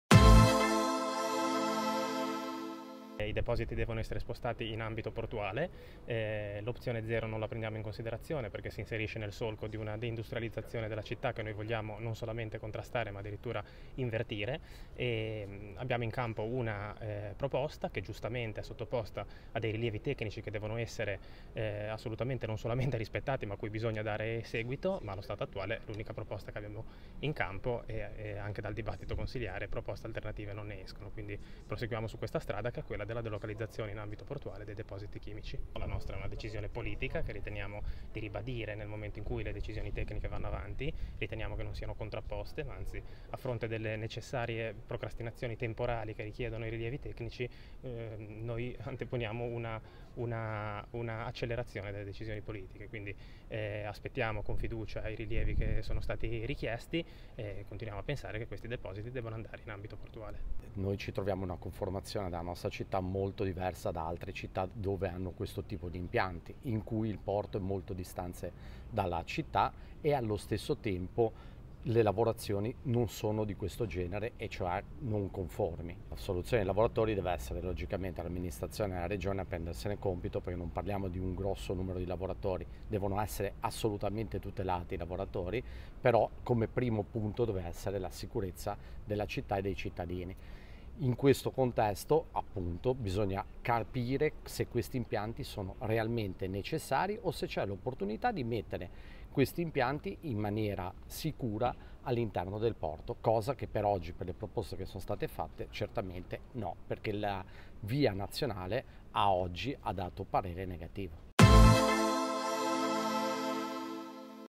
Depositi chimici, scontro in consiglio comunale sull'opzione zero
In ordine:
- Consigliere comunale Paolo Gozzi (Vince Genova)
- Consigliere comunale Fabio Ceraudo (Movimento 5 Stelle)